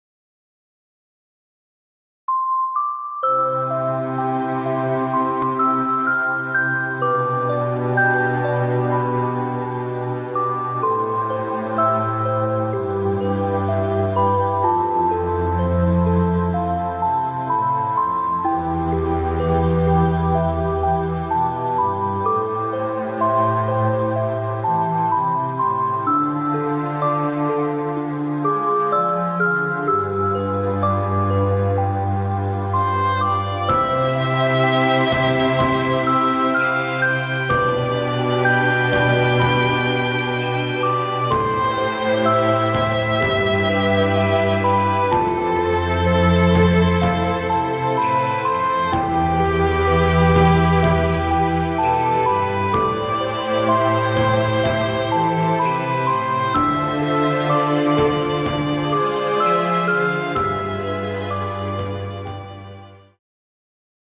2017 仙台トラストシティ イルミネーション
現在、仙台トラストシティで開催中の美しいイルミネーションの様子です。
この動画で使用している音楽は、以下のサイトからお借りしています。